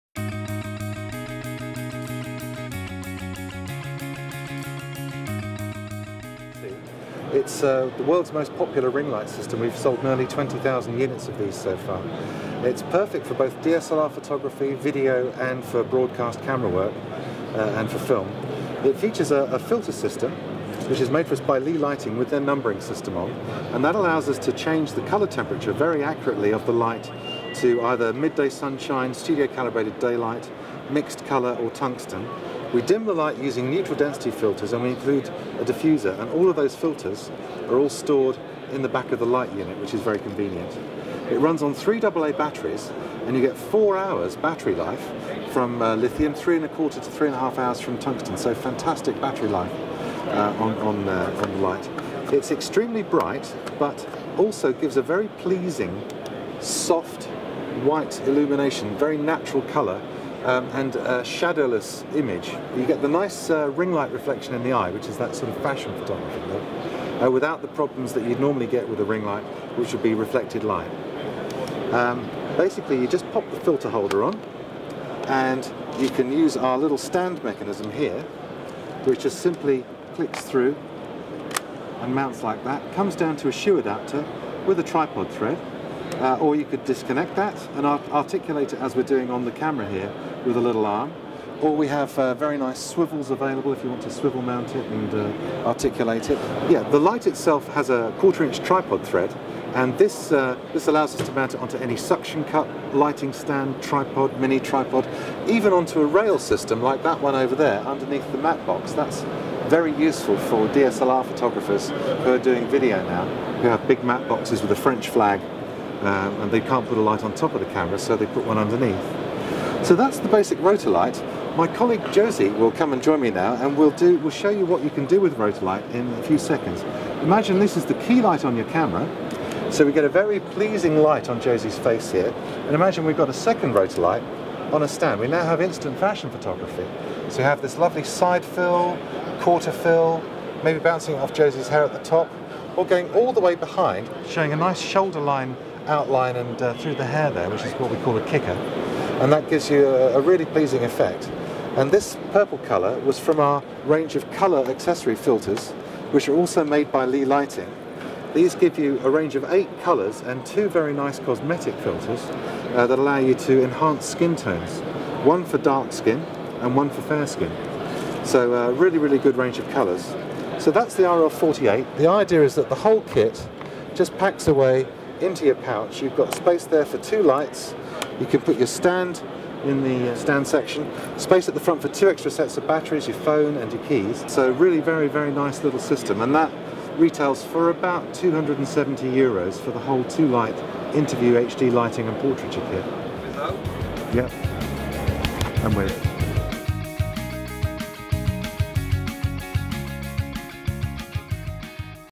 Rotolight - Led Solutions for HDSLR (IBC2011, Amsterdam - RAI)
Rod Gammons, de Rotolight, nous présente son anneau Led, parfaite solution d'éclairage pour les boîtiers reflex numériques (DSLR). Disponible sur notre boutique